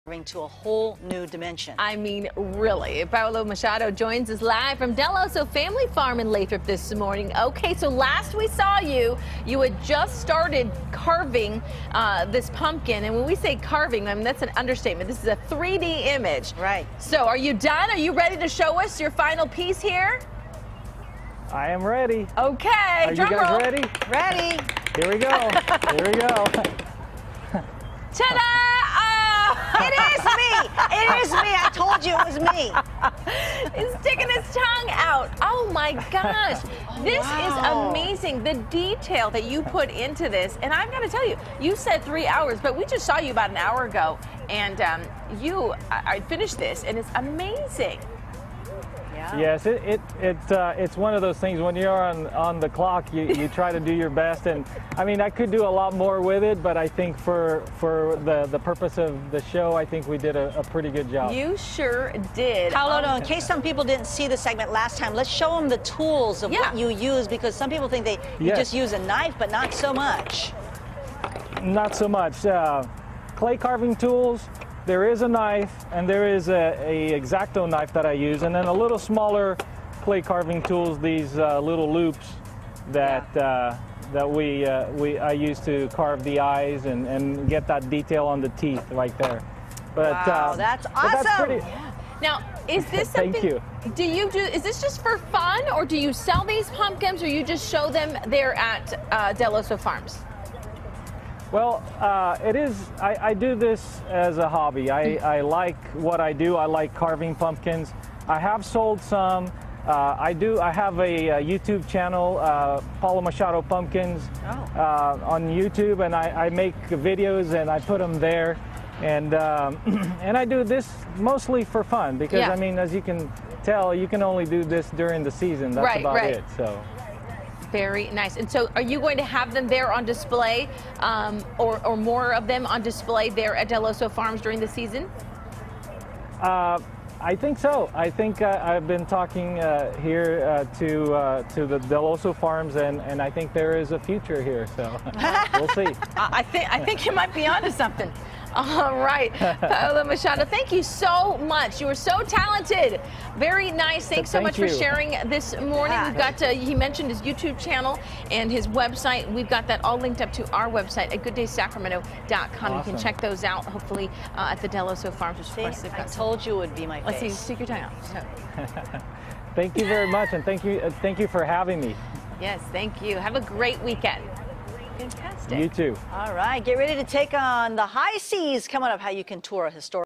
3D Pumpkin Carving Pt. 2
We’re getting a look a pumpkin carving at its finest!